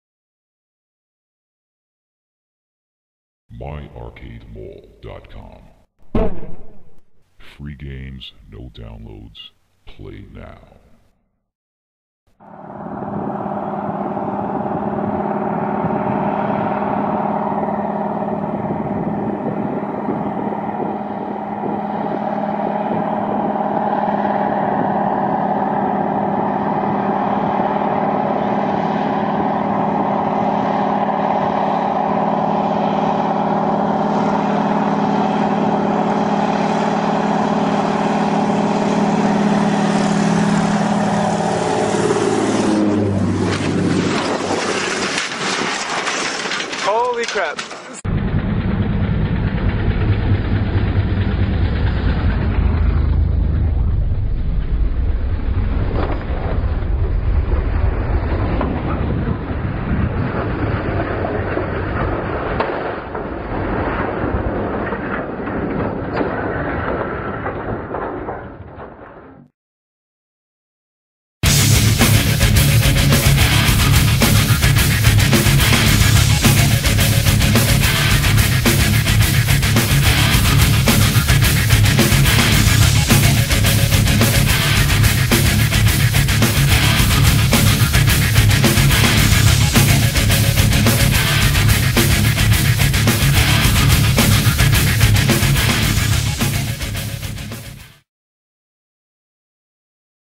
Sea Plane crash
This one is amazing. The man video taping this even nearly got hit by the plane himself